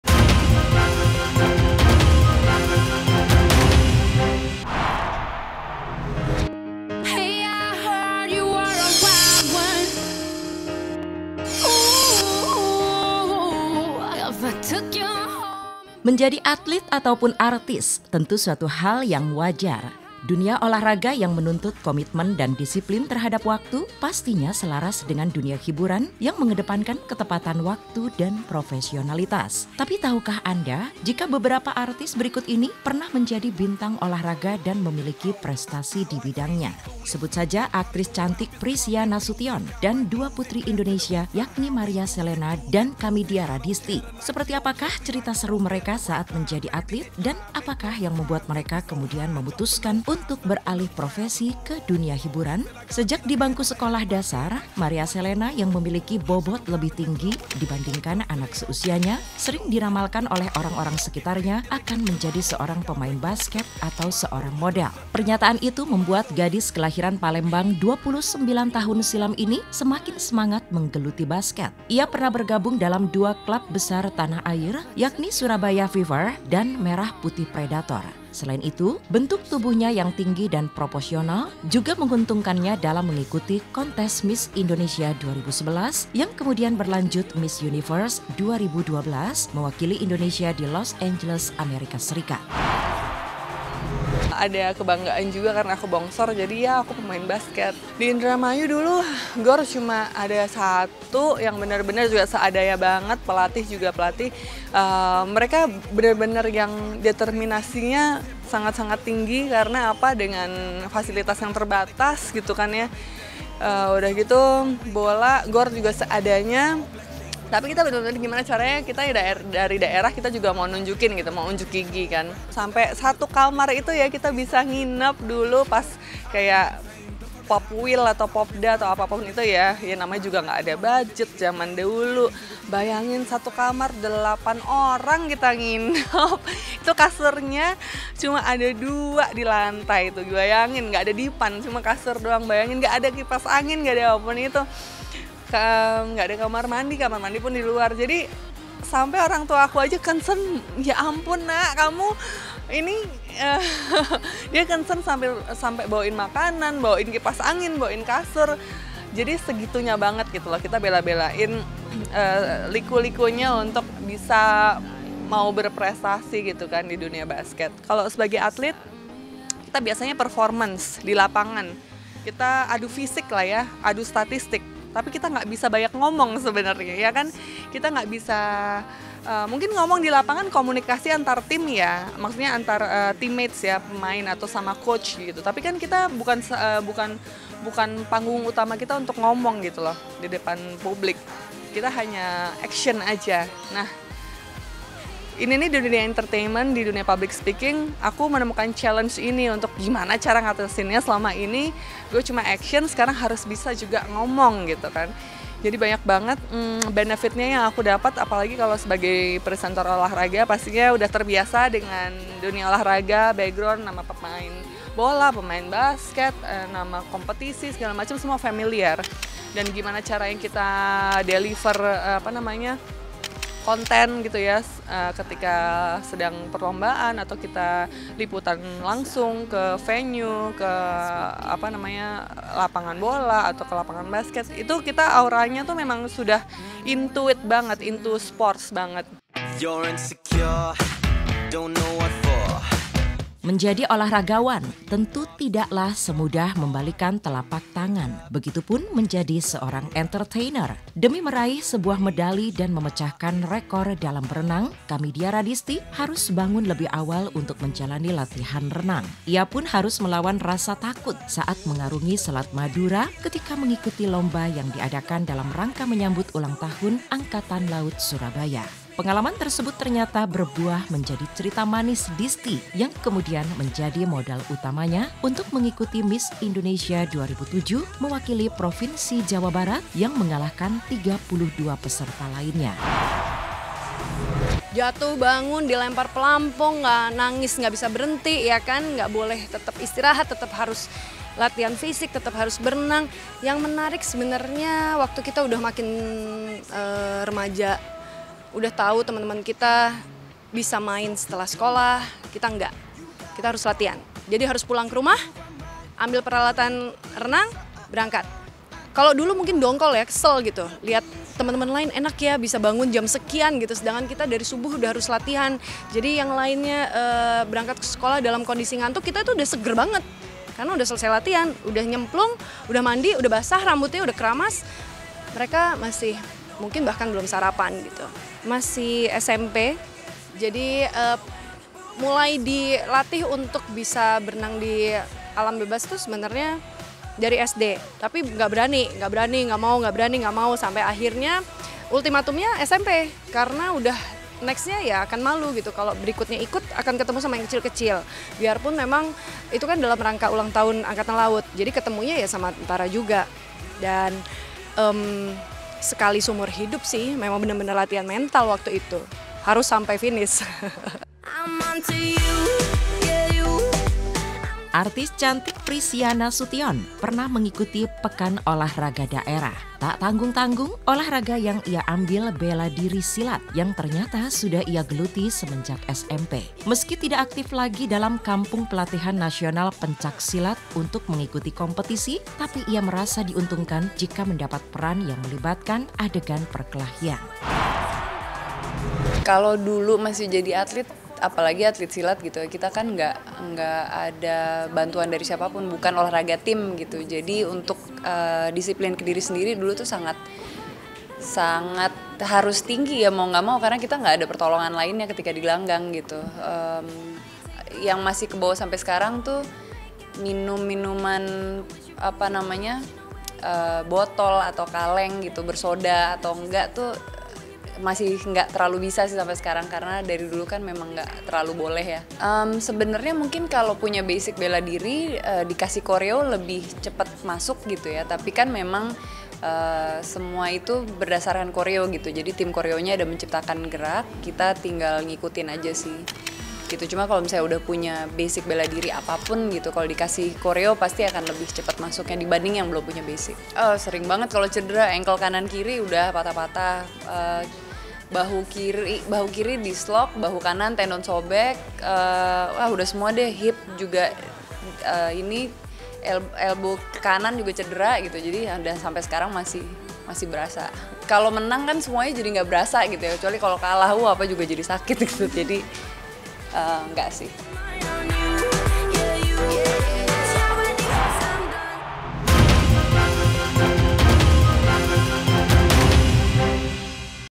"Cerita Artis yang Berasal Dari Atlet"; Prisia Nasution, Kamidia Radisti dan Maria Selena
Yuk gabung digrup Facebook Bintang Mas Entertainment